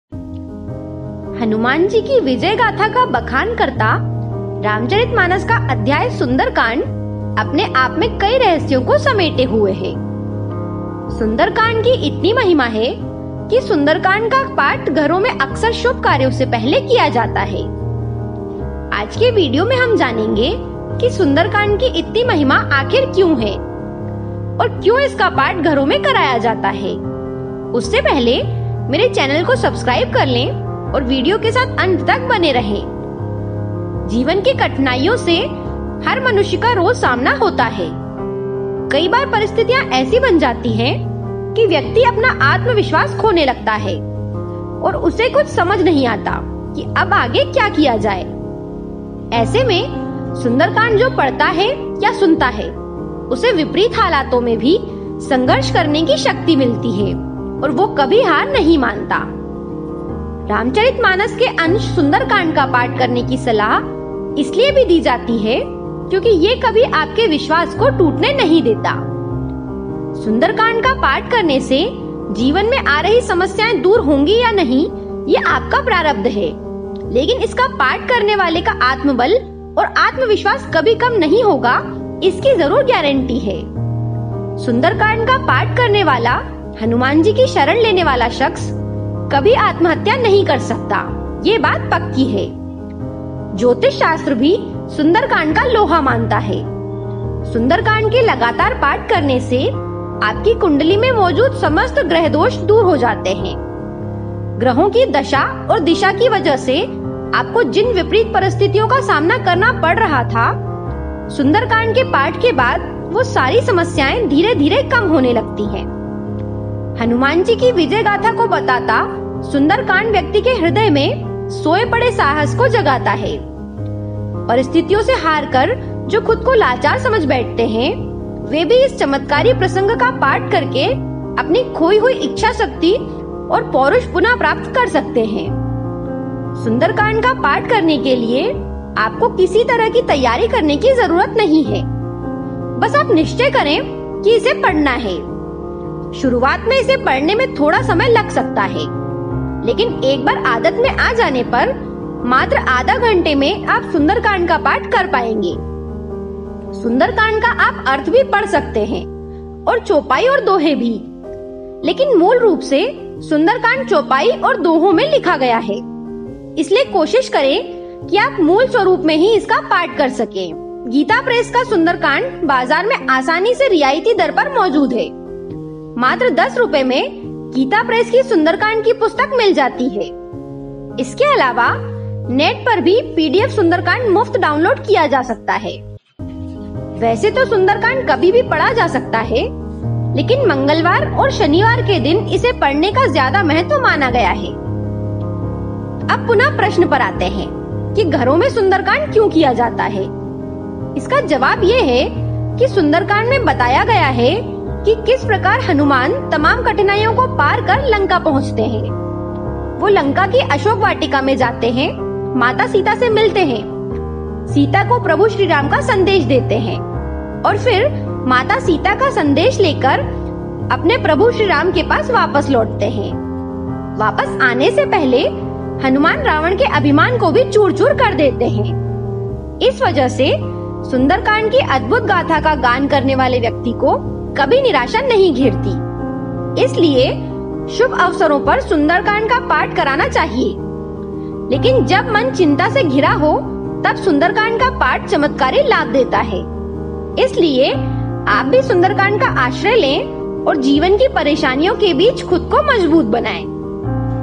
सुन्दरकाण्ड के चमत्कार | Sunderkand kyon kiya Jata Hai | Sunderkand Sunderkand Path ke Chamatkar
हनुमान जी की विजय गाथा का बखान करता रामचरितमानस का अध्याय सुंदरकांड अपने आप मे कई रहस्यों को समेटे हुए है। सुंदरकांड की इतनी महिमा है कि सुंदरकांड का पाठ घरों में अक्सर शुभ कार्य से पहले किया जाता है। आज के वीडियो में हम जानेंगे कि सुंदरकांड की इतनी महिमा क्यों है और क्यों इसका पाठ घरों में कराया जाता है?

जीवन की कठिनाइयों से हर मनुष्‍य का रोज सामना होता है। कई बार परिस्थितियां ऐसी बन जाती हैं कि व्यक्ति अपना आत्‍मविश्‍वास खोने लगता है और उसे कुछ समझ नही आता कि अब आगे क्या किया जाए? ऐसे में सुंदरकांड जो पढ़ता है, सुनता है उसे विपरीत हालातों में भी संघर्ष करने की शक्ति मिलती है और वो कभी हार नही मानता।
रामचरितमानस के अंश सुंदरकांड का पाठ करने की सलाह इसलिए भी दी जाती है क्‍योंकि कभी आपके विश्‍वास को टूटने नहीं देता। सुंदर कांड का पाठ करने से जीवन में आ रहीं समस्‍याएं दूर होंगी या नही ये आपका प्रारब्ध है। लेकिन इसका पाठ करने वाले का आत्मबल और आत्मविश्वास कभी कम नही होगा इसकी जरूर ग्यारंटी है। सुंदरकांड का पाठ करने वाला, हनुमान जी की शरण लेने वाला शख्स कभी आत्महत्या नही कर सकता ये बात पक्की है।

ज्‍योतिष शास्‍त्र भी सुंदरकांड का लोहा मानता हैं। सुंदरकांड के लगातार पाठ करने से आपकी कुंडली में मौजूद समस्‍त ग्रह दोष दूर हो जाते हैं । ग्रहों की दशा और दिशा की वजह से आपको जिन विपरीत परिस्थितियों का सामना करना पड़ रहा था सुंदर कांड के पाठ के बाद वो सारी समस्‍याएं धीरे-धीरे कम होती जाती है।

हुनमान जी की विजय गाथा को बताता सुंदर कांड व्‍यक्ति के हृदय में सोए पड़े साहस को जगाता है। परिस्थितियों से हारकर जो खुद को लाचार समझ बैठते हैं वे भी इस चमत्‍कारी प्रसंग का पाठ करके अपनी खोई हुई इच्‍छाशाक्ति और पौरुष पुनः प्राप्त कर सकते हैं।

सुंदरकांड का पाठ करने के लिए आपको किसी तरह की तैयारी की जरूरत नहीं है बस आप निश्चय करें कि इसे पढ़ना है। शुरुवात में इसे पढ़ने में थोड़ा समय लग सकता है लेकिन एक बार आदत में आने पर मात्र आधे घंटे में आप सुंदरकांड का पाठ कर पाएंगे। सुंदरकांड का आप अर्थ भी पढ़ सकतें है और चौपाई, दोहे भी। लेकिन मूल रूप से सुंदरकांड चैपाई और दोहों में लिखा गया है इसलिये कोशिश करें कि आप मूल स्वरूप में ही इसका पाठ कर सकें। गीताप्रेस का सुंदरकांड बाज़ार में आसानी से रियायती मूल्य पर मिल जाता है। मात्र 10 रुपये में गीताप्रेस की सुंदरकांड की पुस्तक मिल जाती है। इसके अलावा नेट पर भी पीडीएफ सुंदरकांड मुफ्त डाऊनलोड किया जा सकता है।
वैसे तो सुंदरकांड कभी भी पढ़ा जा सकता है लेकिन मंगलवार और शनिवार के दिन इसे पढ़ने का ज्यादा महत्व माना जाता है।
अब प्रश्न पर आते है कि घरों में सुंदरकांड क्यों किया जाता है?
इसका जवाब ये है कि सुंदरकांड में बताया गया है कि किस प्रकार हनुमान तमाम कठिनाइयों को पार कर लंका पहुंचते हैं । वो लंका की अशोक वाटिका में जाते हैं,माता सीता से मिलते हैं । सीता को प्रभु श्री राम का संदेश देते हैं और फिर माता सीता का संदेश लेकर वापस अपने प्रभु श्री राम के पास लौटतै हैं । वापस आने से पहले हनुमान रावण के अभिमान को भी चूर-चूर कर देते है।

इस वजह से सुंदरकांड की अद्भुत गाथा का गान करने वाले व्‍यक्ति को कभी निराशा नहीं घेरती। इसलिए शुभ अवसरों पर सुंदरकांड का पाठ करना चाहिए लेकिन जब मन चिंता से घिरा हो तब सुन्दरकाण्ड का पाठ चमत्कारी लाभ देता है। इसलिए आप भी सुन्दरकाण्ड का आश्रय लें और जीवन की परेशानियों के बीच खुद को मजबूत बनाएं।
Sundara Kaanda is the fifth book in the Hindu epic, the Ramayana. The original Sundara Kanda is in Sanskrit and was composed by Valmiki, who was the first to scripturally record the Ramayana. Sundara Kanda is the only chapter of the Ramayana in which the hero is not Rama, but rather Hanuman. The work depicts the adventures of Hanuman and his selflessness, strength, and devotion to Rama are emphasized in the text. Hanuman was fondly called “Sundara” by his mother Anjani and Sage Valmiki chose this name over others as the Sundara Kanda is about Hanuman's journey to Lanka.
हनुमान जी ने लंका की ओर प्रस्थान किया। सुरसा ने हनुमान जी की परीक्षा ली और उसे योग्य तथा सामर्थ्यवान पाकर आशीर्वाद दिया। मार्ग में हनुमान जी ने छाया पकड़ने वाली राक्षसी का वध किया और लंकिनी पर प्रहार करके लंका में प्रवेश किया। उनकी विभीषण से भेंट हुई। जब हनुमान जी अशोकवाटिका में पहुँचे तो रावण सीता को धमका रहा था। रावण के जाने पर त्रिजटा ने सीता को सान्त्वना दी। एकान्त होने पर हनुमान जी ने सीता से भेंट करके उन्हें राम की मुद्रिका दी। हनुमान जी ने अशोकवाटिका का विध्वंस करके रावण के पुत्र अक्षय कुमार का वध कर दिया। मेघनाथ हनुमान को नागपाश में बांध कर रावण की सभा में ले गया। रावण के प्रश्न के उत्तर में हनुमान ने अपना परिचय राम के दूत के रूप में दिया। रावण ने हनुमान जी की पूँछ में तेल में डूबा हुआ कपड़ा बांध कर आग लगा दिया इस पर हनुमान जी ने लंका का दहन कर दिया। हनुमान जी सीता के पास पहुँचे। सीता ने अपनी चूड़ामणि दे कर उन्हें विदा किया। वे वापस समुद्र पार आकर सभी वानरों से मिले और सभी वापस सुग्रीव के पास चले गये। हनुमान के कार्य से राम अत्यंत प्रसन्न हुये। राम वानरों की सेना के साथ समुद्रतट पर पहुँचे। उधर विभीषण ने रावण को समझाया कि राम से बैर न लें इस पर रावण ने विभीषण को अपमानित कर लंका से निकाल दिया। विभीषण राम के शरण में आ गया और राम ने उसे लंका का राजा घोषित कर दिया। राम ने समुद्र से रास्ता देने की विनती की। विनती न मानने पर राम ने क्रोध किया और उनके क्रोध से भयभीत होकर समुद्र ने स्वयं आकर राम की विनती करने के पश्चात् नल और नील के द्वारा पुल बनाने का उपाय बताया।